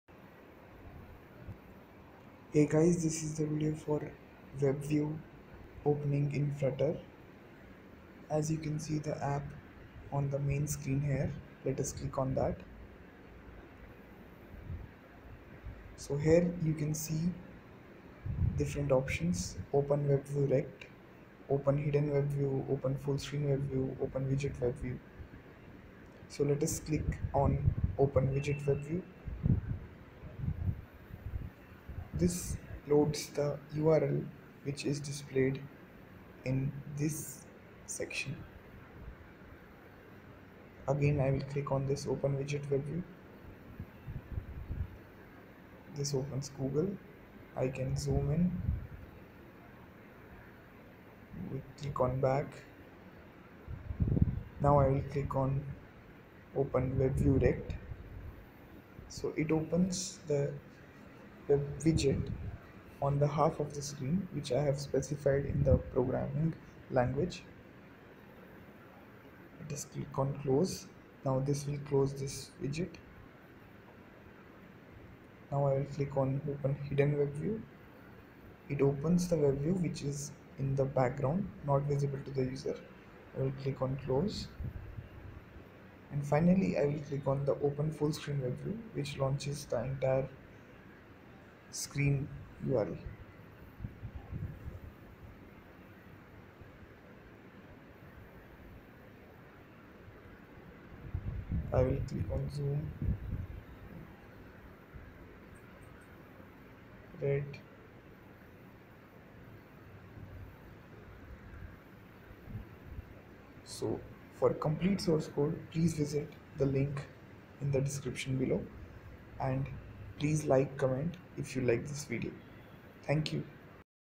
WebView in Flutter | Using WebView in Flutter | WebViews in Flutter @aseemwangoo
This video shows the use of webview in the flutter. We can listen to the URL changes by the user.

* WebView in Flutter | Using WebView in Flutter *
Going through a variety of articles/plugins related to Flutter, I came across this plugin, named WebView.
As a developer, we may face a situation where we would require to open a URL in the application itself. Well, this plugin is an answer to all those questions.
Let’s Begin…
import flutter_webview_plugin into the pubspec.yaml

and then include,
import ‘package:flutter_webview_plugin/flutter_webview_plugin.dart’; into your dart file. Now, you will be able to avail of the features of this plugin…
Next…You will need to use the WebViewScaffold

Parameters :
    url: The URL which you want to open.
    withZoom: Allow the users to zoom the webpage.
    withLocalStorage: Saving data in the device storage.